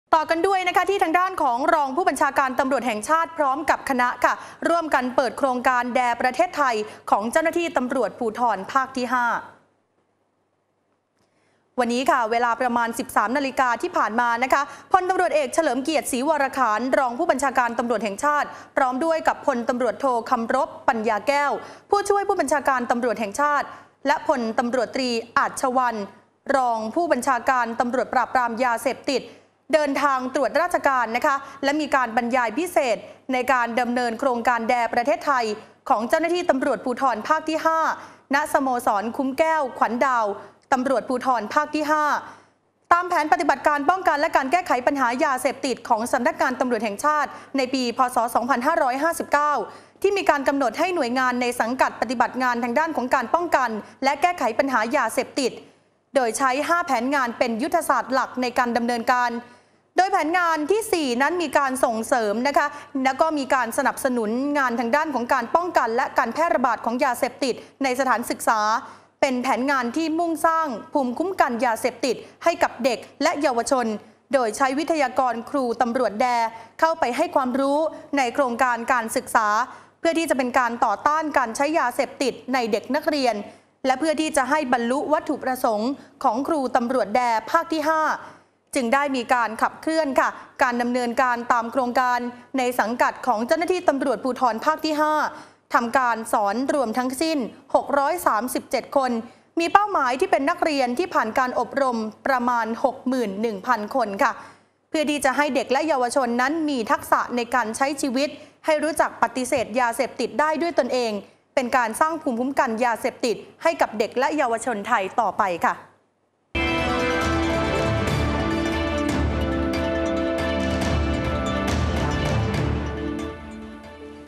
เปิดโครงการแดร์ ตำรวจภูธร ภาค5
3 มีนาคม 2559 เวลา13:00น.
พล.ต.อ.เฉลิมเกียรติ ศรีวรขาน รอง ผบ.ตร. พร้อมด้วย พล.ต.ท.คำรบ ปัญญาแก้ว ผู้ช่วยผบ.ตร. และ พล.ต.ต.อาชวันต์ โชติกเสถียร รองผู้บัญชาการตำรวจปราบปรามยาเสพติด เดินทางตรวจราชการ และบรรยายพิเศษ ในการดำเนินการโครงการD.A.R.E. ประเทศไทย ของตำรวจภูธรภาค5 ณ สโมสรคุ้มแก้ว-ขวัญดาว ตำรวจภูธรภาค5
***เพื่อให้เด็กและเยาวชนมีทักษะในการใช้ชีวิต ให้รู้จักปฎิเสธยาเสพติดได้ด้วยตนเอง เป็นการสร้างภูมิคุ้มกันยาเสพติดให้กับเด็กและเยาวชนไทยต่อไป

PoliceTV